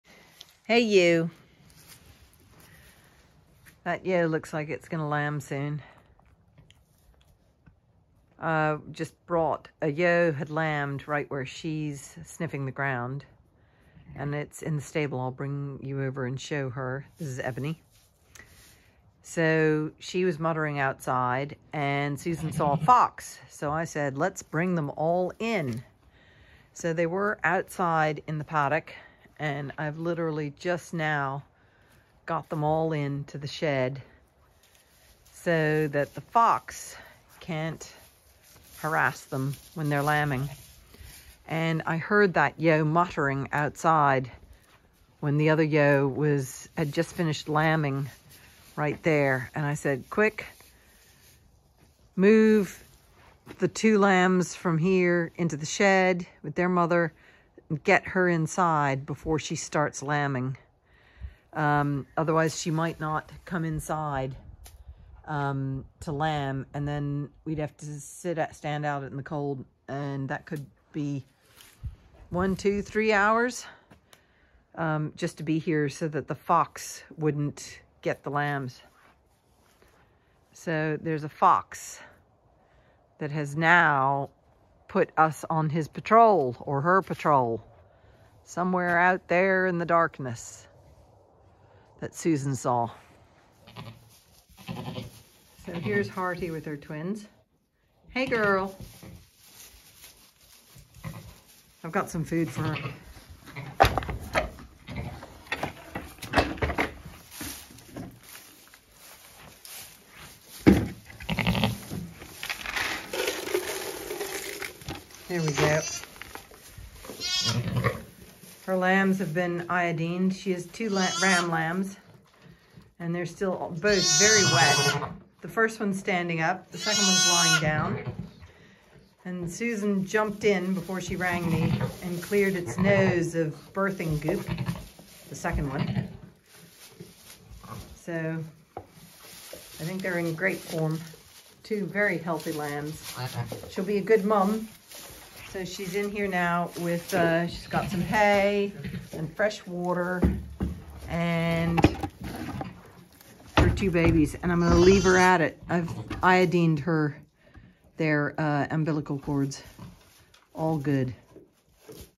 Midnight the witching hour, more lambs born & a fox makes an appearance
I hustle ewes into shed before the next ewe starts to seriously lamb. Clever foxes can really hassle a ewe when she’s giving birth. So it’s much easier to eliminate the possibility by putting ewes back into shed out of harm’s way. Mr fox can go hunt up a rat, vole or mouse instead.